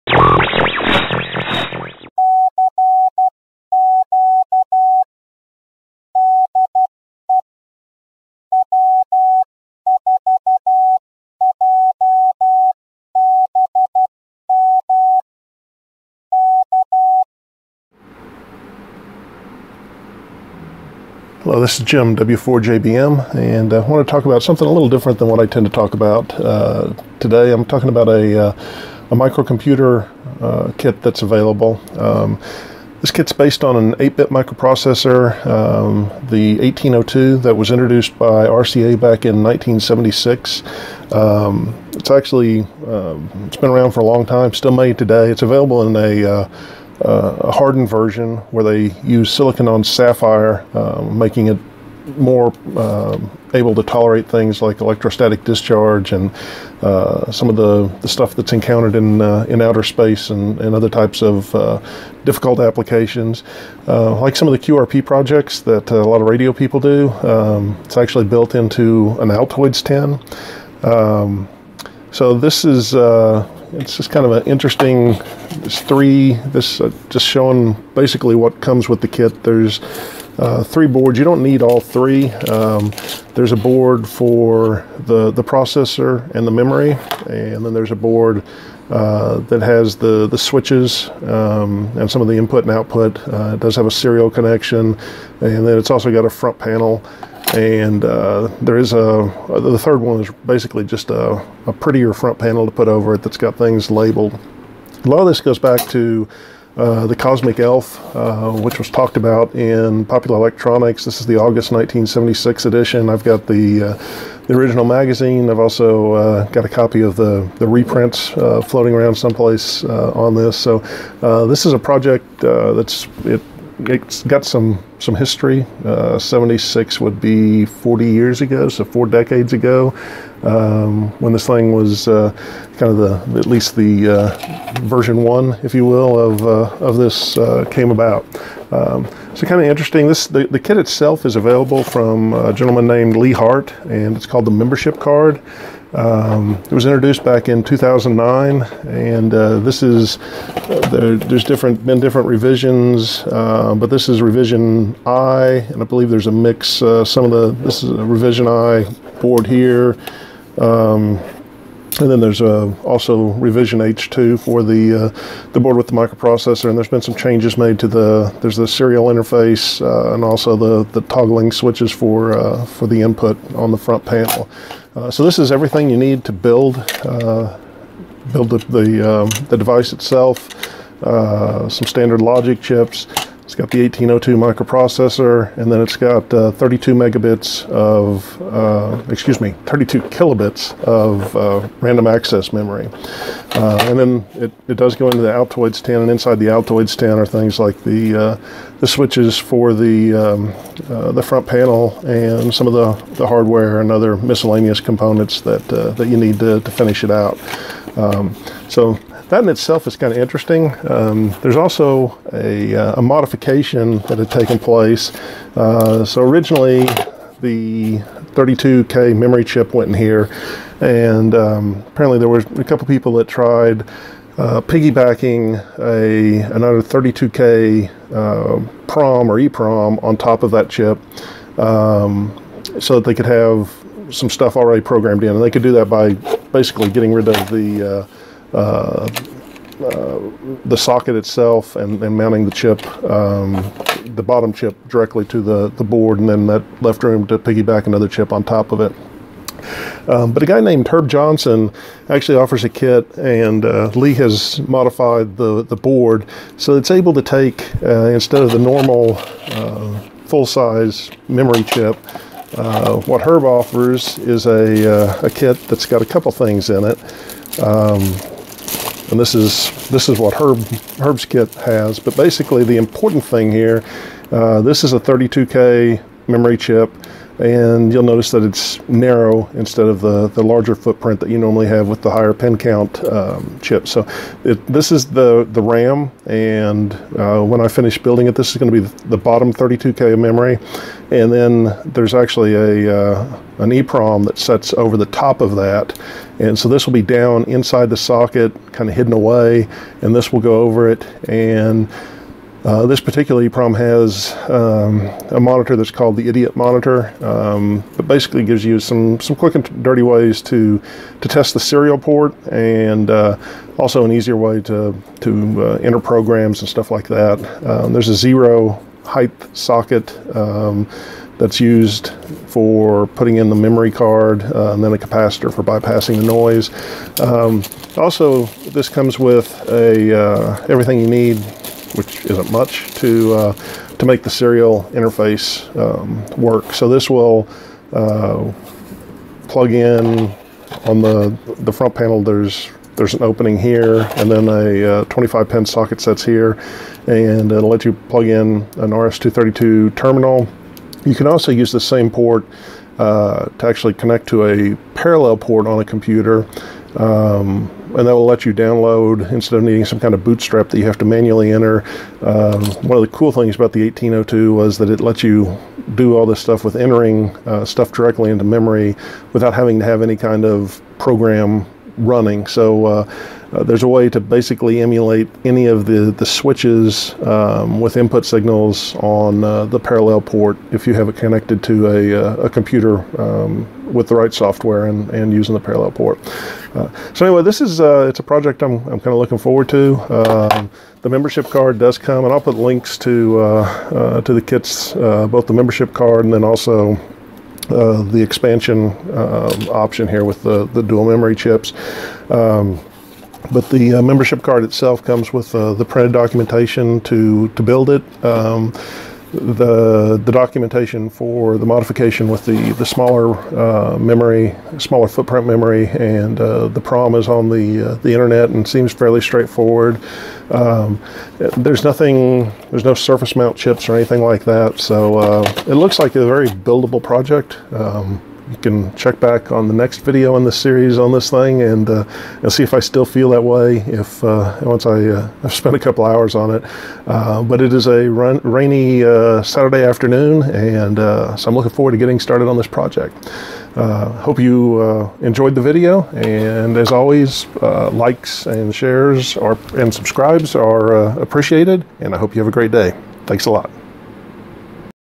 The 1802 Microprocessor and the Membership Card kit
Have you ever had the urge to build your own computer? If so, now's your chance.

The 1802 is a vintage microprocessor that has been used in everything from home computers to industry control systems to NASA space probes. It is still made today and now you can "roll your own" system based on one.

Herb Johnson's kit that adds up to 32K of ROM while letting you keep a full 32K of RAM as well as adding a monitor and enabling the serial interface can be found at:

A blast from the past for those of use who grew up in the 1970s and 1980s and microcomputers were just emerging and a chance to experience a little piece of microcomputing history for everyone!